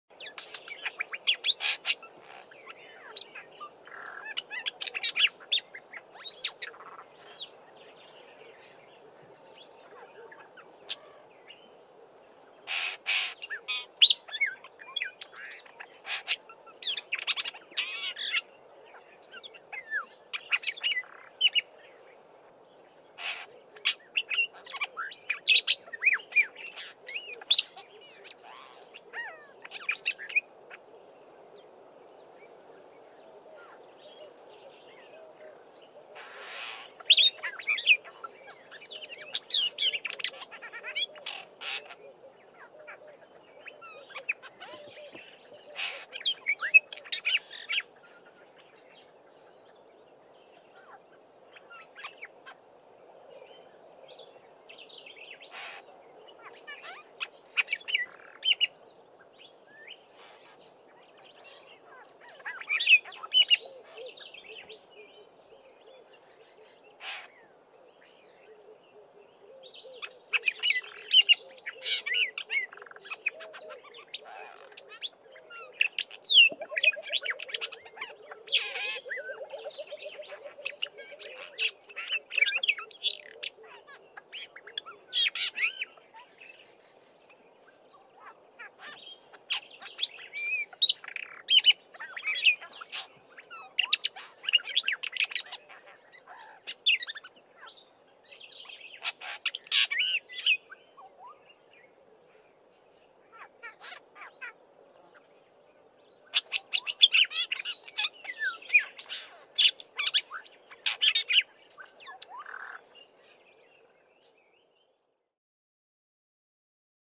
Black-bellied Starling singing and mimicking
Listen how this Black-bellied Starling sings and throws in a lot of mimicry of a Fork-tailed Drongo and some other local birds.